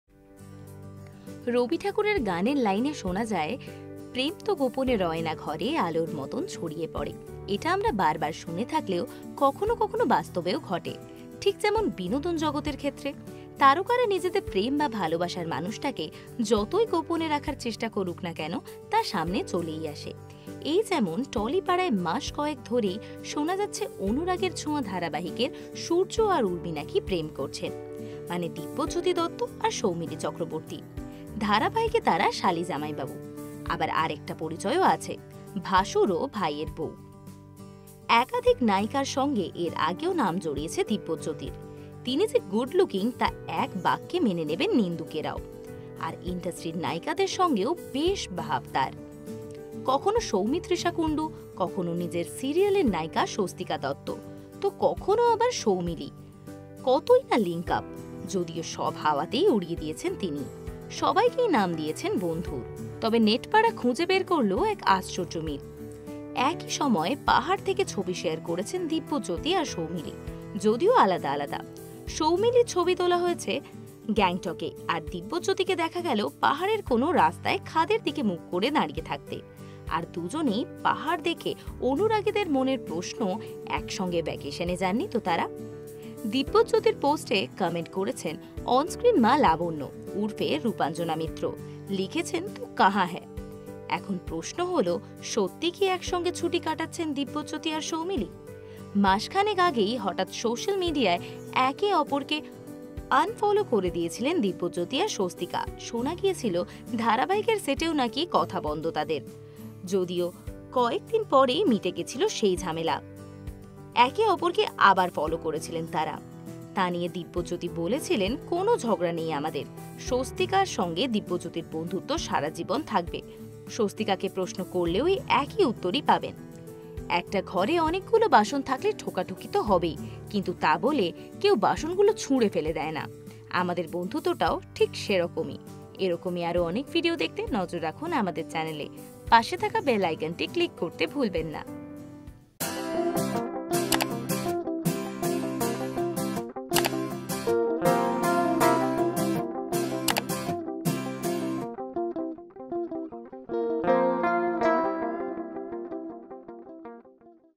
প্রেম করছেন অনুরাগের ছোঁয়ার সূর্য আর উর্মি Anurager Chhowa Dibyojyoti Dutta Soumily Chakraborty
তারকারা নিজেদের প্রেম বা ভালোবাসার মানুষটাকে যতই গোপনে রাখার চেষ্টা করুক না কেন, তা সামনে চলেই আসে।এই যেমন টলিপাড়ায় মাসকতক ধরেই শোনাজাচ্ছে ,অনুরাগের ছোঁয়া ধারাবাহিকের সূর্য আর উর্মি নাকি প্রেম করছেন! মানে দিব্যজ্যোতি দত্ত আর সৌমিলি চক্রবর্তী।

Are Anurager Chhowa Stars Dibyojyoti Dutta and Soumily Chakraborty Vacationing Together?